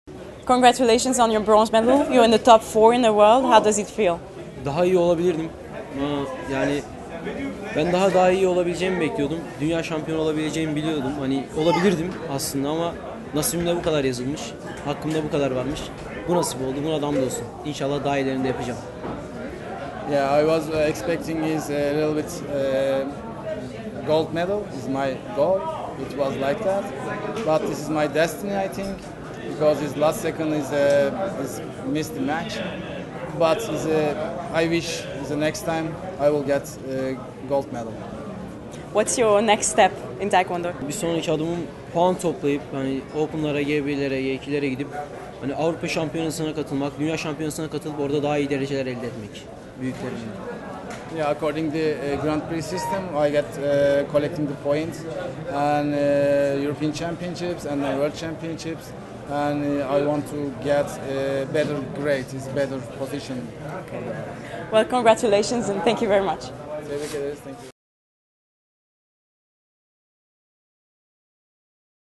[INTERVIEW] BRONZE | KAVURAT Ferhat Can (TUR, Male –63kg)
2016 WORLD JUNIOR TAEKWONDO CHAMPIONSHIPS